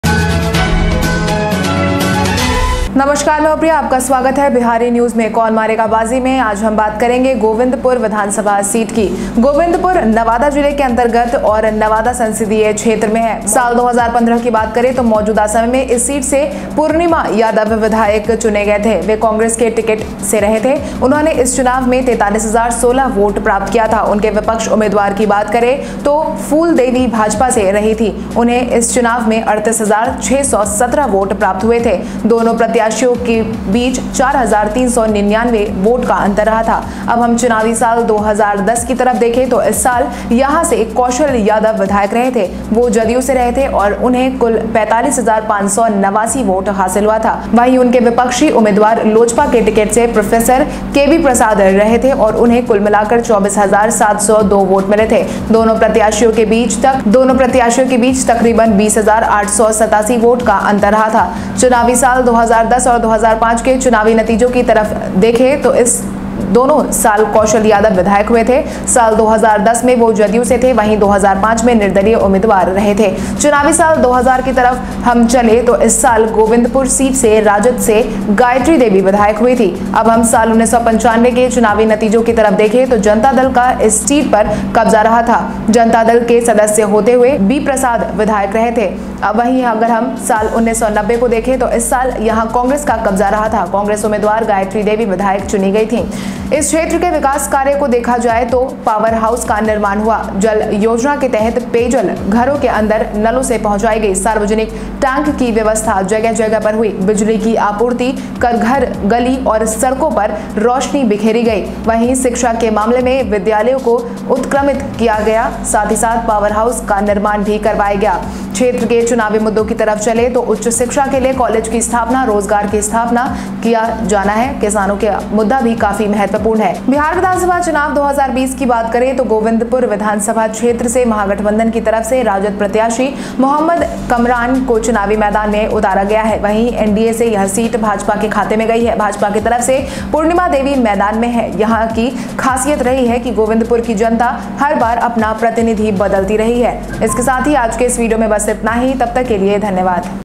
Govindpur vidhansabha: कौन मारेगा बाजी गोविंदपुर So far MLA in Govindpur 2015,2010,2005,2000,1995.
Govindpur is under Nawada district and in Nawada parliamentary constituency.
Year 2015
Currently, PurnimaYadav was elected MLA from this seat. He was from Congress ticket. He received 43 thousand 16 votes in this election. Talking about their opposition candidate, PhoolDevi was from BJP. He received 38 thousand 6 hundred 17 votes in this election. There was a difference of 4 thousand 3 hundred 99 votes between the two candidates.
Year 2010
Now if we look at the election year 2010, Kaushal Yadav was an MLA from here this year. He was from JDU and received a total of 45 thousand 5 hundred 89 votes. At the same time, his opposition candidate was Pro KB Prasad from LJP ticket and he got a total of 24 thousand 7 hundred 2. There was a difference of about 20 thousand 8 hundred 87 votes between the two candidates.
Year 2010 and 2005
Turning to the election results of the election year 2010 and 2005, Kaushal Yadav was an MLA in both these years. In 2010, he was from JDU, while in 2005 he was an independent candidate.
Year 2000
If we go towards election year 2000, this year Gayatri Devi was MLA from RJD from Govindpur seat.
Year 1995
 Now if we look at the election results of the year 1995, this seat was occupied by Janata Dal. KB Prasad was a legislator while being a member of the Janata Dal.
Year 1990
At the same time, if we look at the year 1990, this year the Congress was occupied here. Congress candidate Gayatri Devi was elected MLA.
development work
If we look at the development work of this area, then the power house was built. Under the water scheme, drinking water was transported through drains inside the houses. Public tanks were arranged all over the place. Light was scattered on the streets of the house and streets by supplying electricity. At the same time, schools were uprooted in terms of education. Pawar House was constructed.

Electoral issues
If we go towards the electoral issues of the region, then the establishment of a college for higher education, employment is to be established. The issue of farmers is also very important.

In 2015, Purnima Yadav was elected MLA from Congress. In 2010, Kaushal Yadav was an MLA from JDU. He became an Independent MLA in 2005. Gayatri Devi was MLA from RJD in the year 2000 while KB Prasad became MLA from Janata Dal in 1995. Gayatri Devi was elected MLA from Congress in 1990.
Talking about Bihar assembly elections 2020, RJD candidate MohammadKamran has been fielded from Govindpur assembly constituency on behalf of the grand alliance. In the NDA, this seat has gone to the BJP's account. Poornima Devi is in the fray from BJP. The specialty of this place is that the people of Govindpur change their representatives every time.